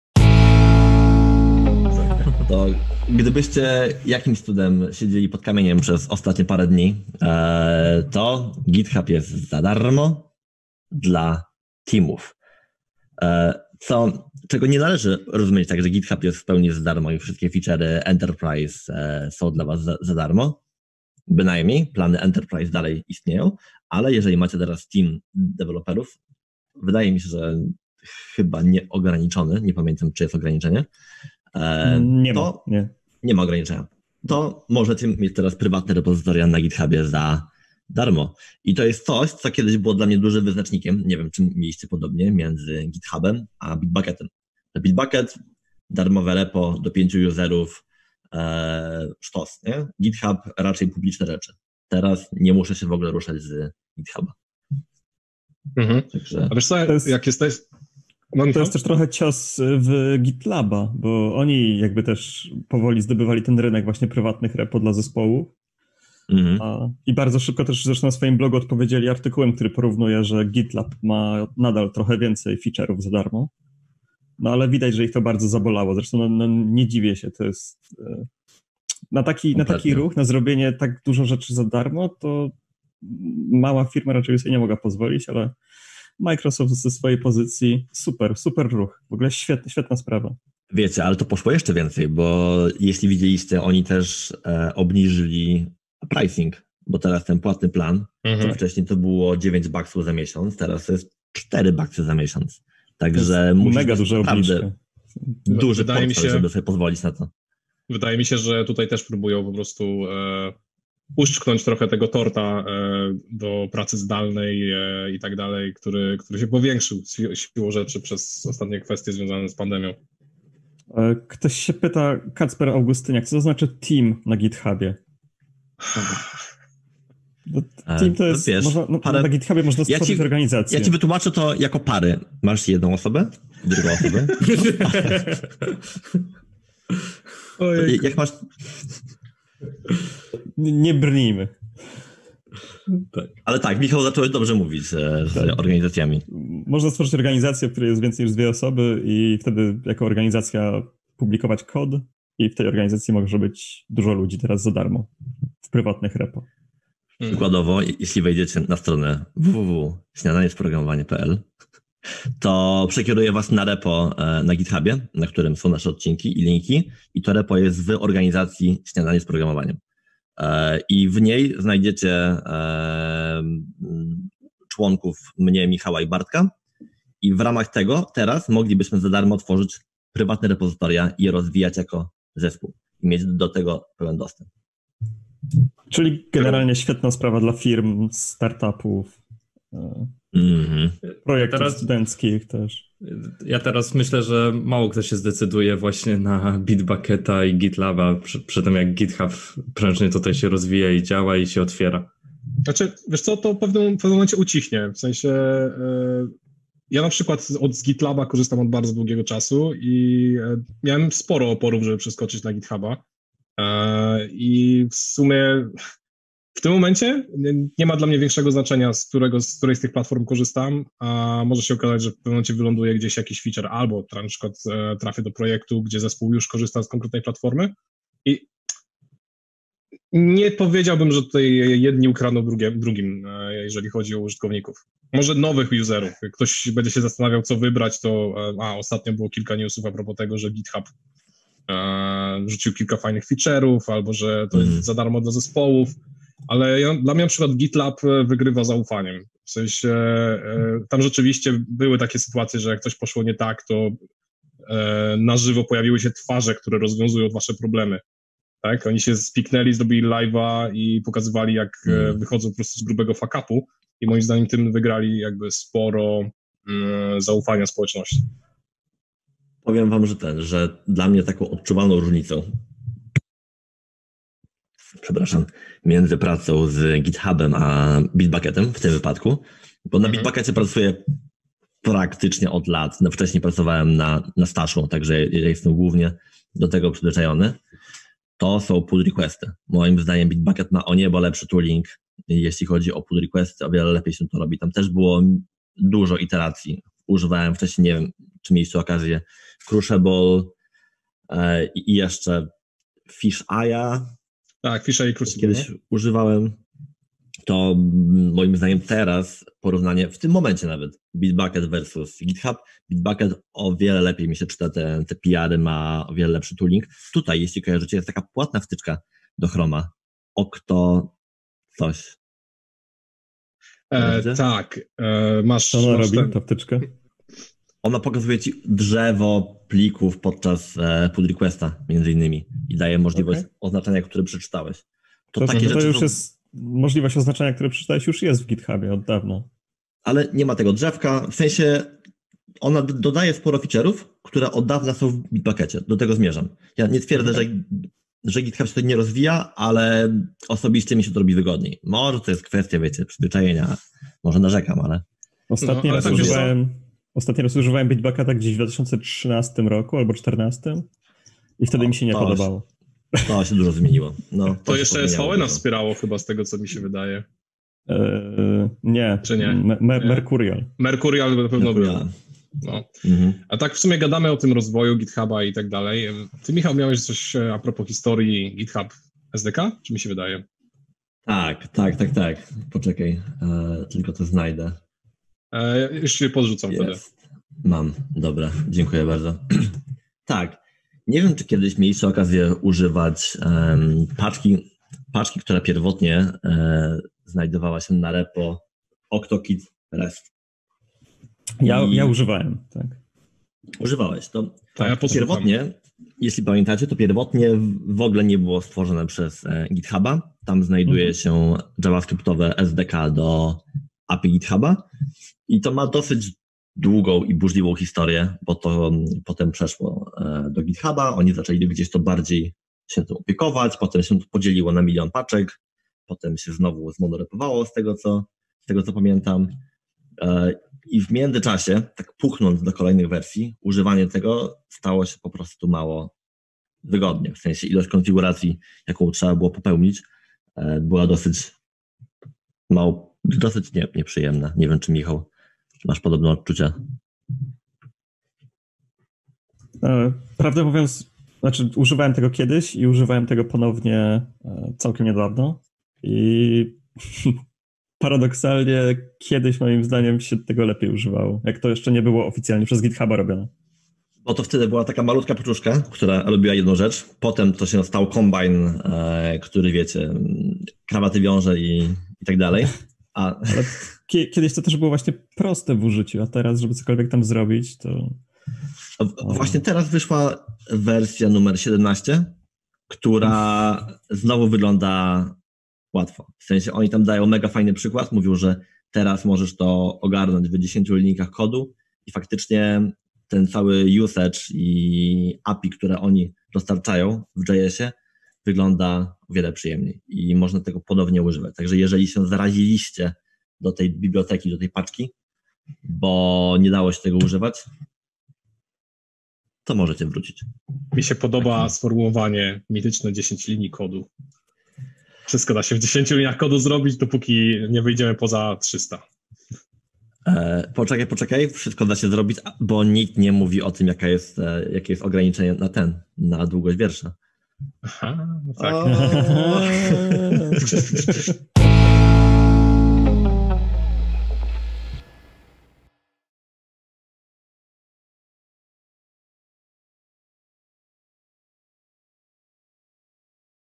GitHub darmowy dla Teamów! | Śniadanie z Programowaniem
W trakcie ostatniego Śniadania z Programowaniem rozmawialiśmy trochę więcej o GitHubie, jego historii, funkcjonalnościach oraz porównaniu z innym serwisem - Bitbucket.